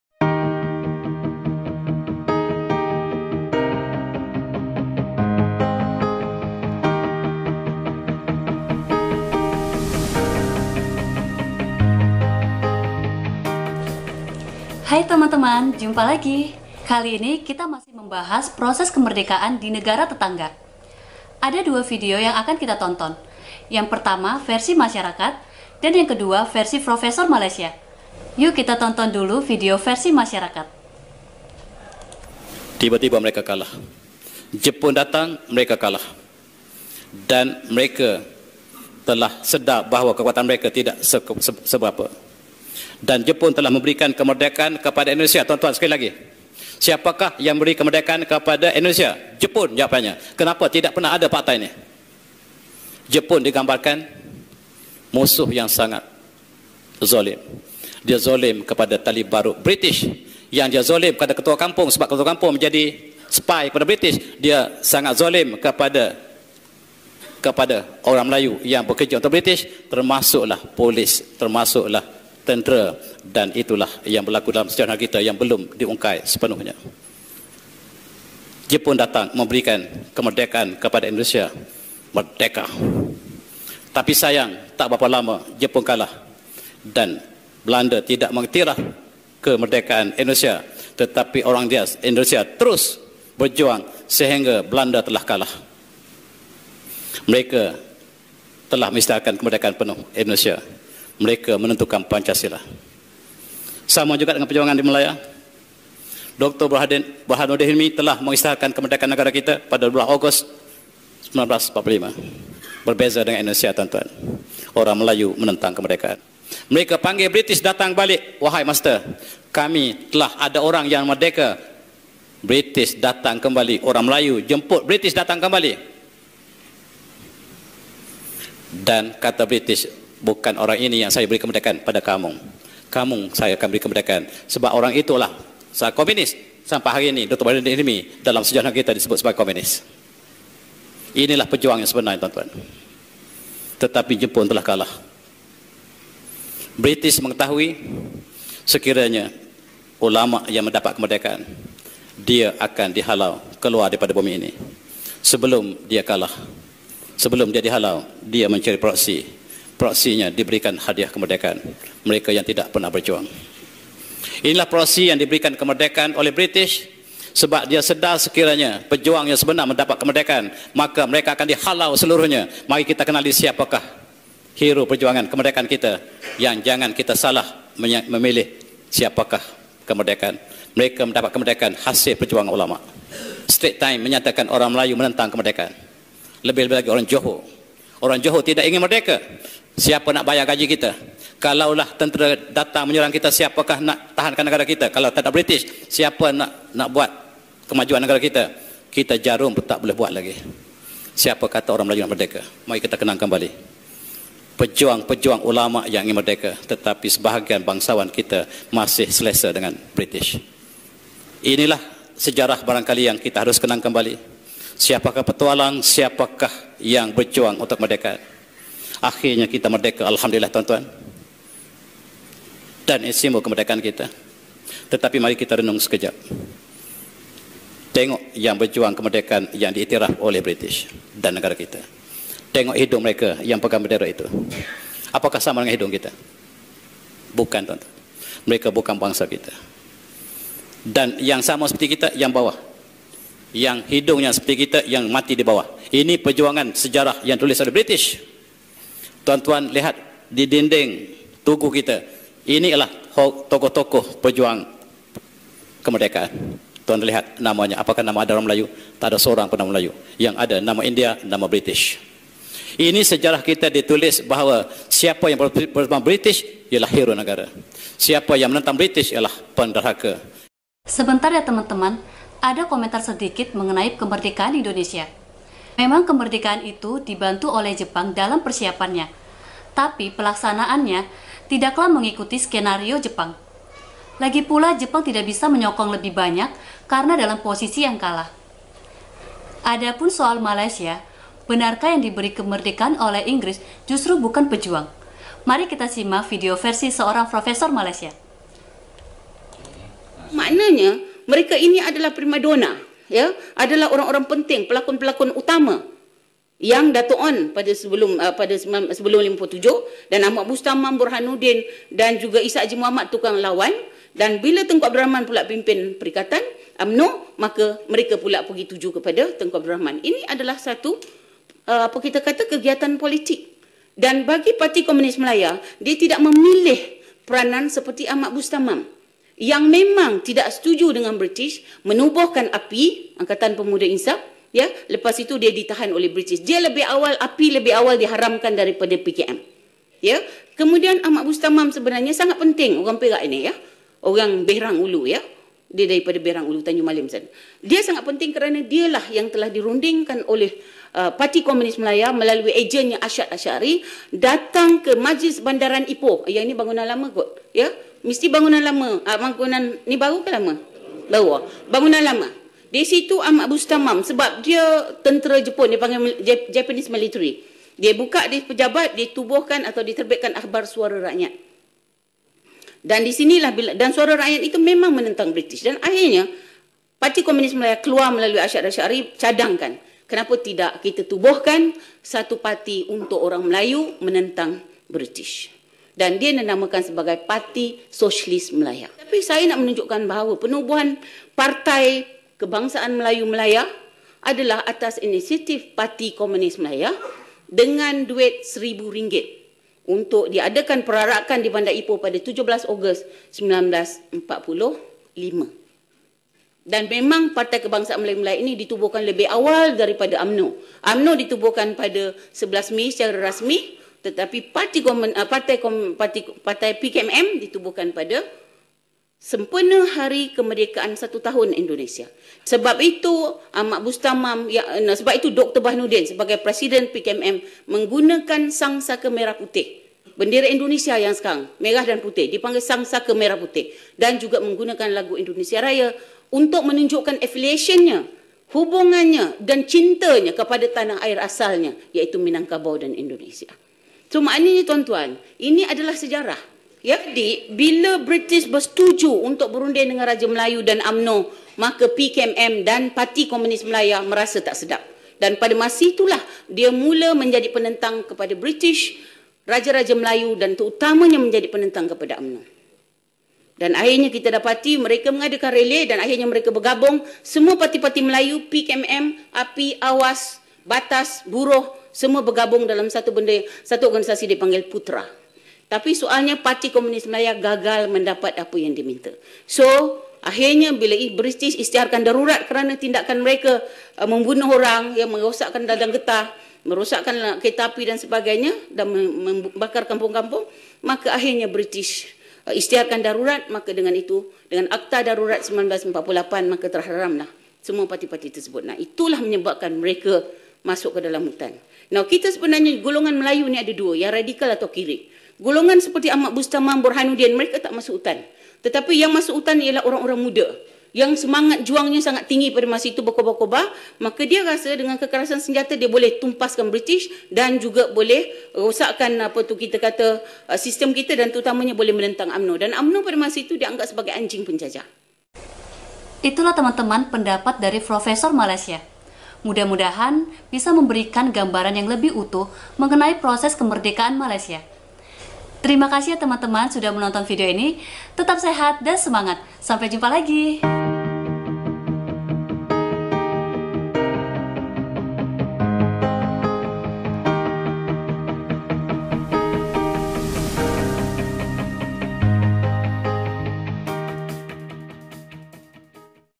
Pahlawan Sejati Malaysia, Berbendera Merah Putih? Dipenjara Karena Melawan Inggris.
Mereka penentang Inggris, pejuang sejati, pahlawan sejati.